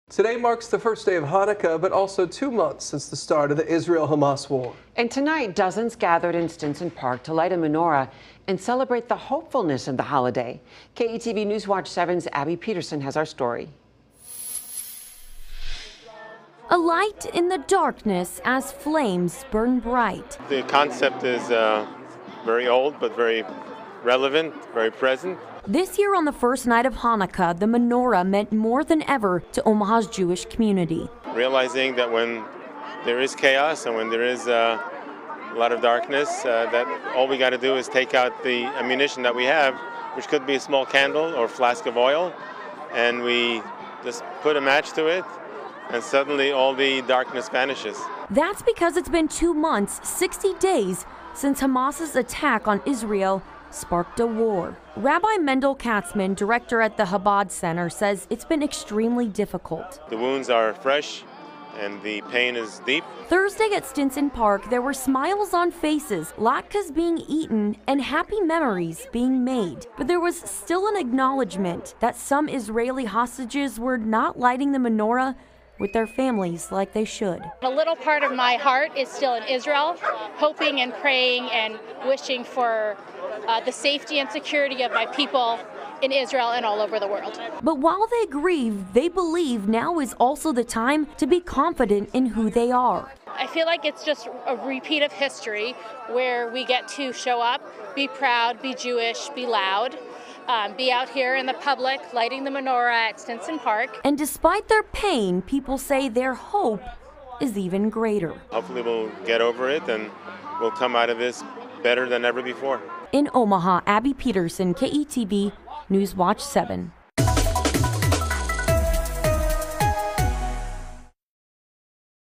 Omaha's Jewish community celebrates Hanukkah at Stinson Park with menorah lighting
Thursday marked the first day of Hanukkah but also two months since the start of the Israel-Hamas war.